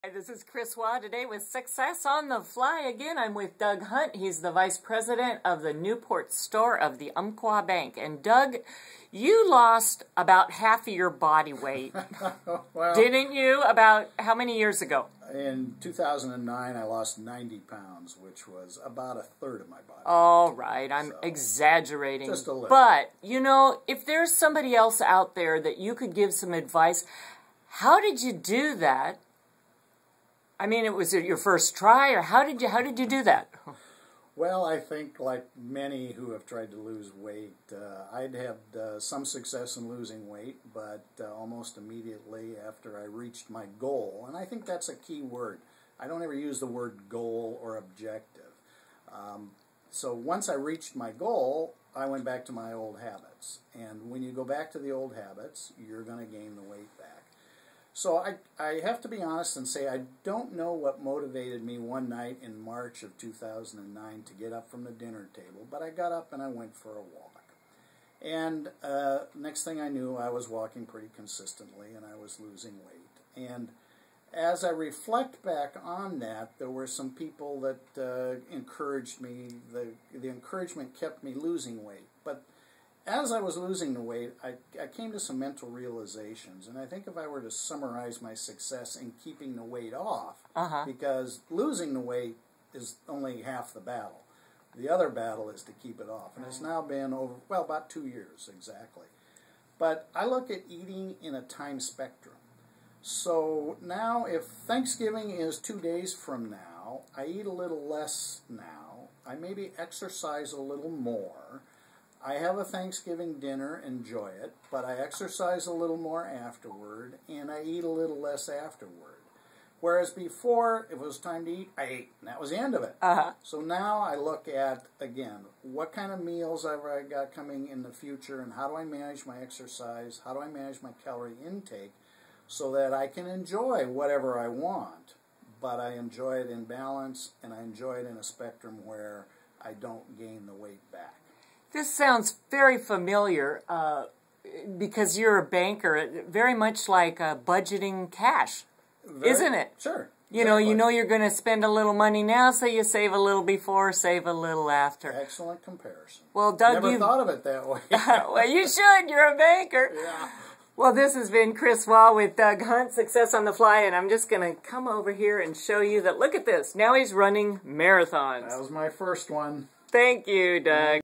Doug Hunt.mpg
Doug Hunt, VP of Umpqua Bank, relays how he managed to lose 90 pounds, and has kept it off for two years.